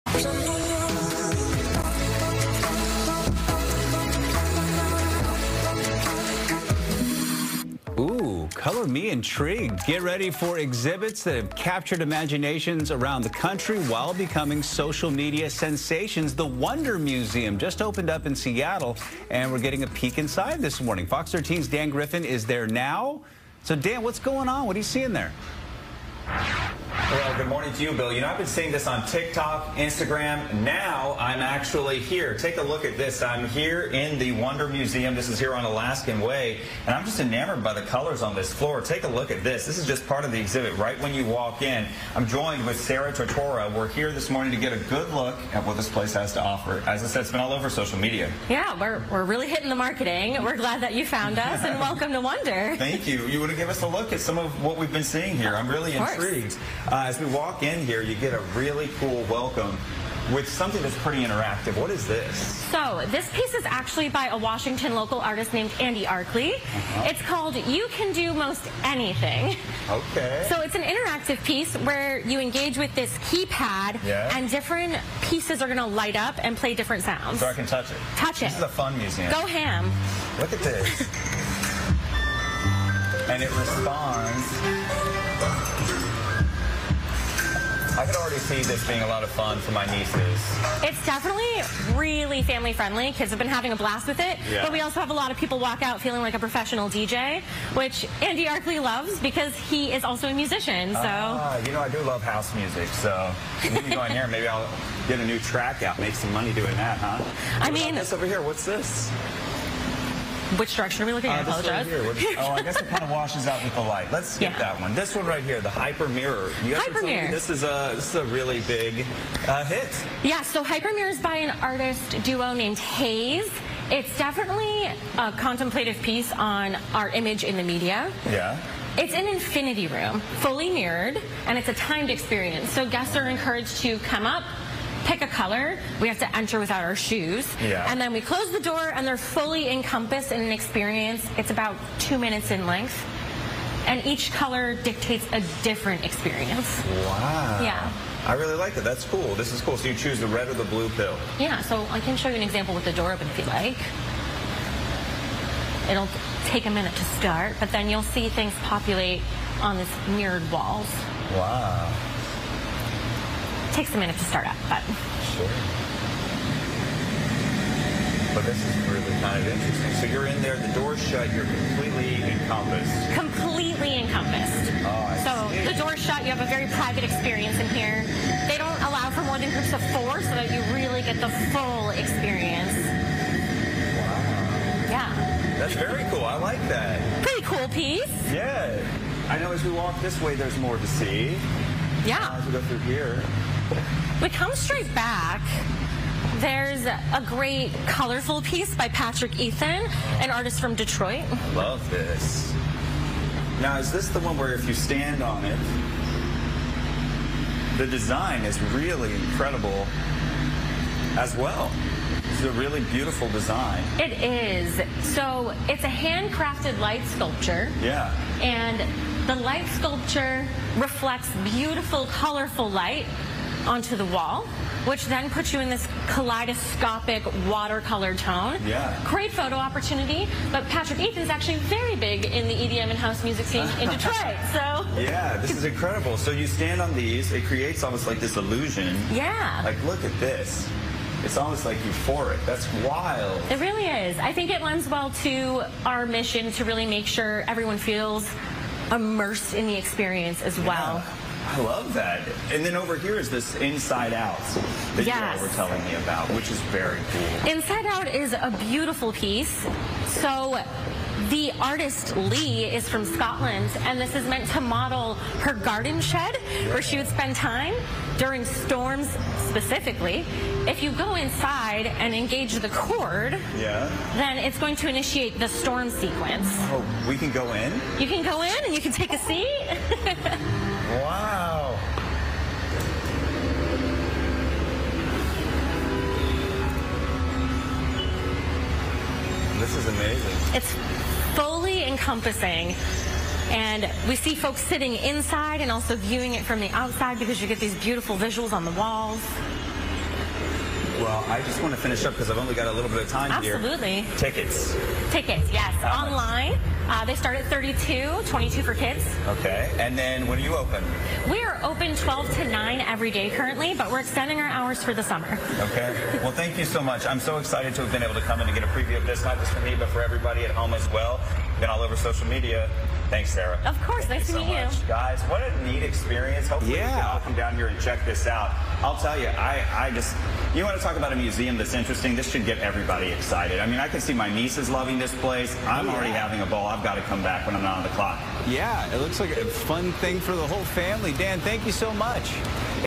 A peek inside Seattle's WNDR Museum
Get ready for exhibits that have captured imaginations across the country. While becoming social media sensations, the WNDR Museum just opened in Seattle.